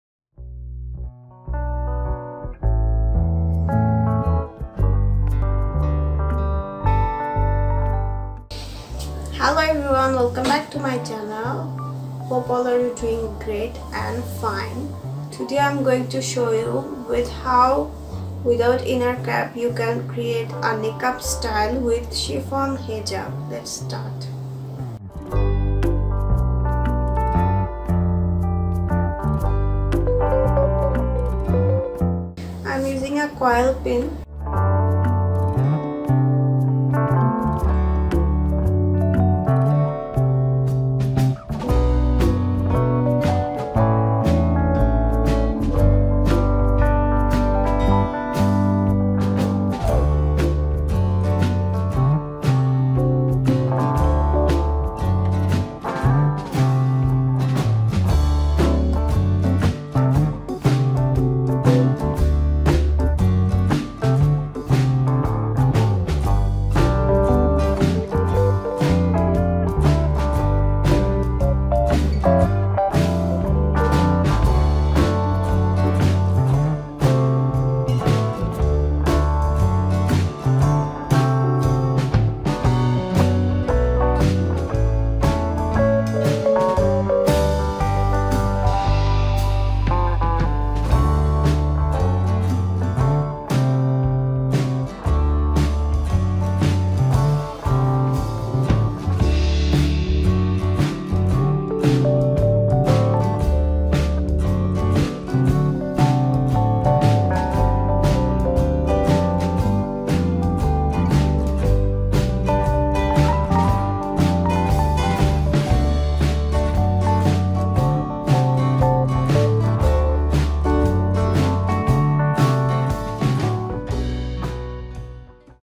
Summer Special Niqab Style Hijab Tutorial without inner Cap ❤️ Easy Niqab Style Tutorial ❤️
Hallow Loves ❤️
Hope all are you doing fine❤️ In this Tutorial I have shown a very easy and summer Friendly Hijab & Niqab Style for you Guys❤️
Hope you will Love this Tutorial ❤️❤️❤️